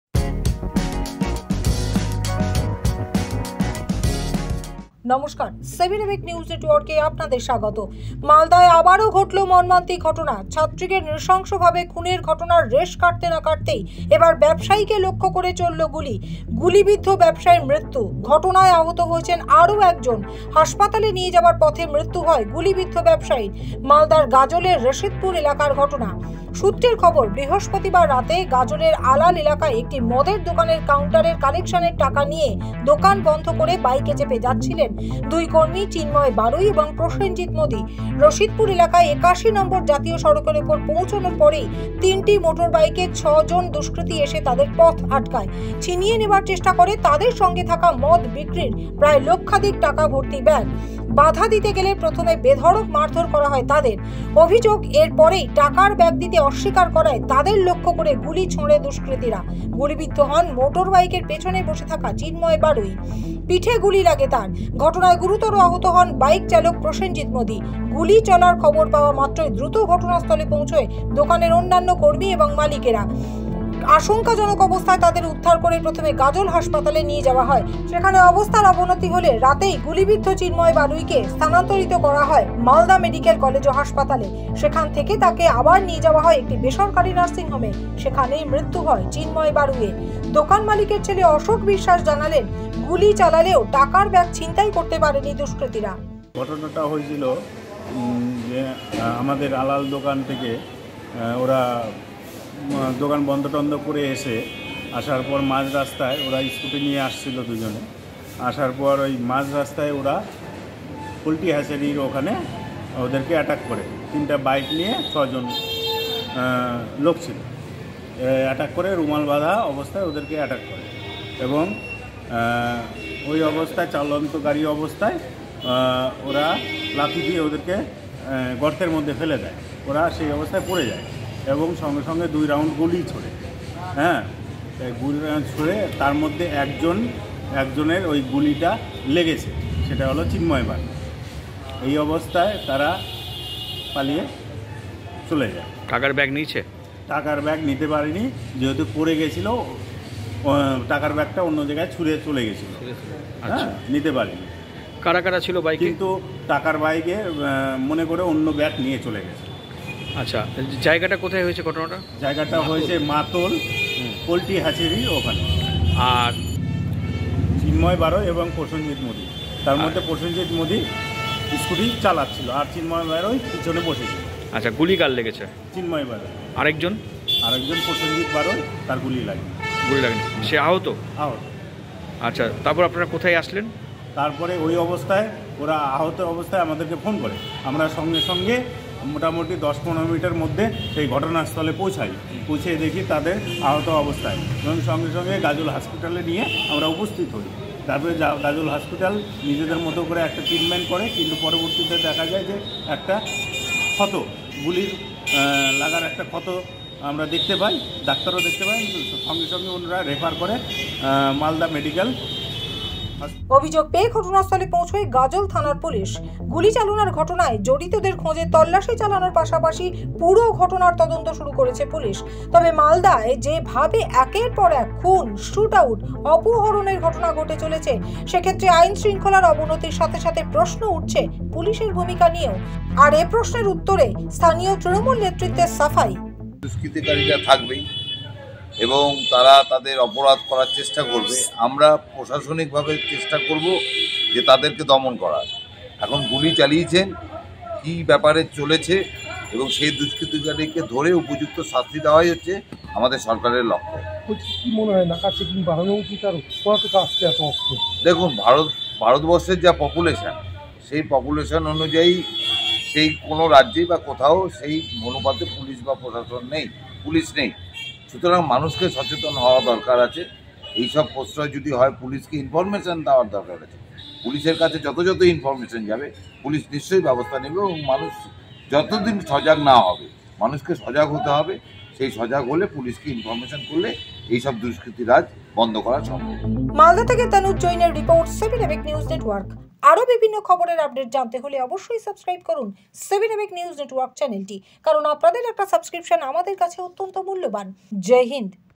Maldah Incident : ফের মালদায় শুট আউট, গুলিবিদ্ধ ১ ব্যবসায়ীর মৃত্যু, ঘটনায় আহত আরো এক ব্যক্তি
ফের মালদায় শুট আউট, গুলিবিদ্ধ ১ ব্যবসায়ীর মৃত্যু, ঘটনায় আহত আরো এক ব্যক্তি

Jai HInd!